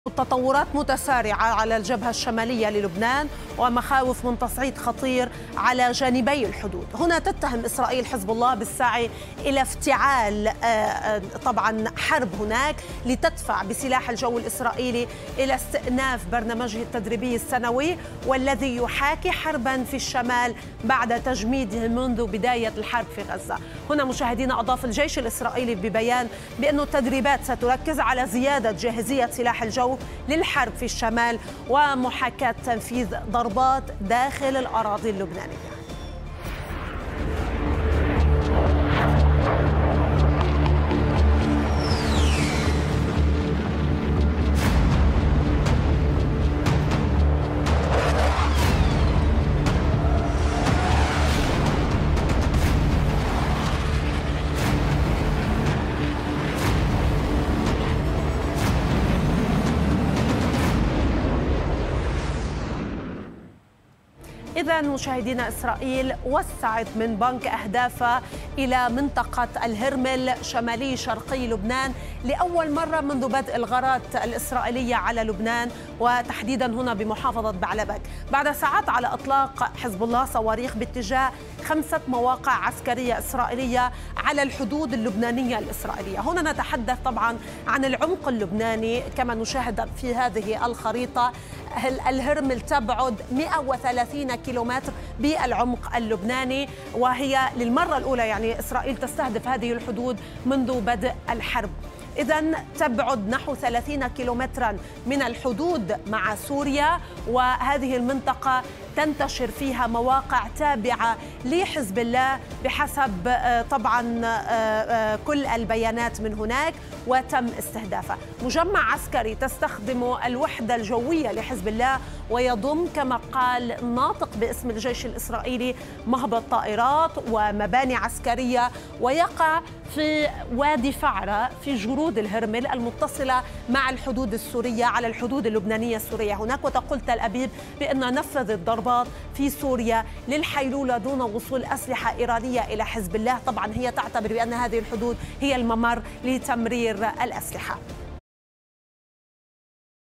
مخاوف دولية من مواجهة مفتوحة في لبنان.. ومناورة إسرائيلية استعدادا لـ"حرب الشمال"
مخاوف دولية من مواجهة مفتوحة في لبنان بعد قصف مجمع عسكري لحزب الله بالهرمل.. ومناورة جوية إسرائيلية استعدادا لـ"حرب الشمال"
لمعرفة أحدث المستجدات.. عبر البث المباشر من هنا ⤵
وبإمكانكم متابعة العربية على منصات التواصل